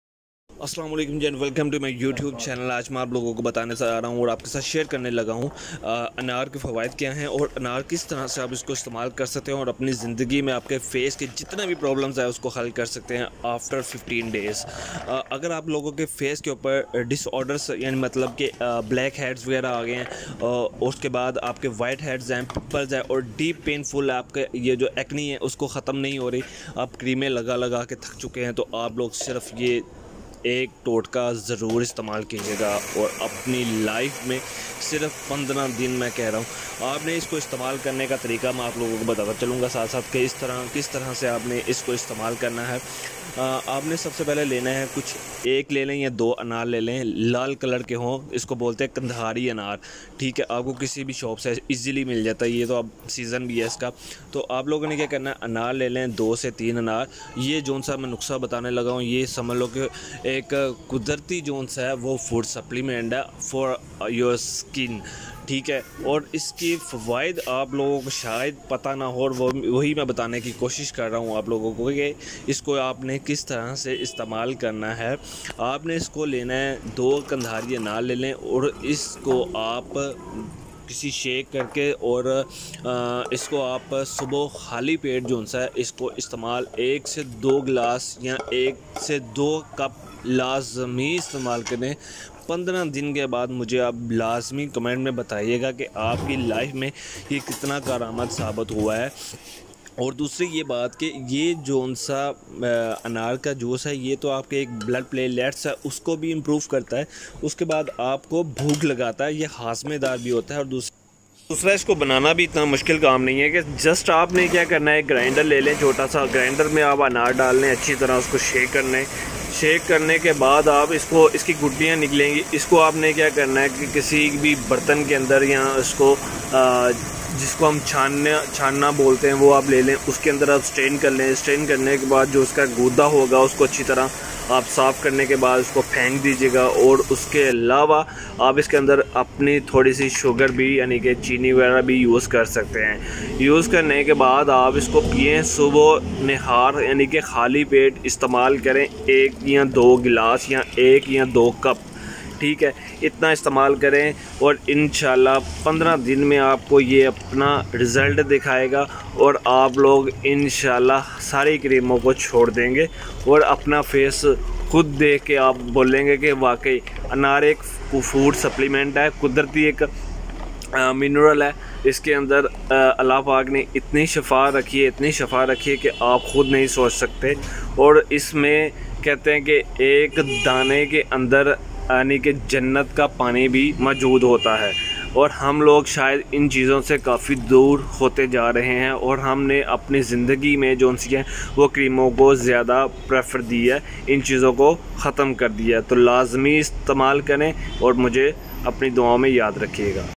Anaar Juice | Health |Face Acne
Today I will tell you about face problems solution
face darkcirucle
1=acne
2=blackheads and
3=whiteheads
4=pimples
5= blood cells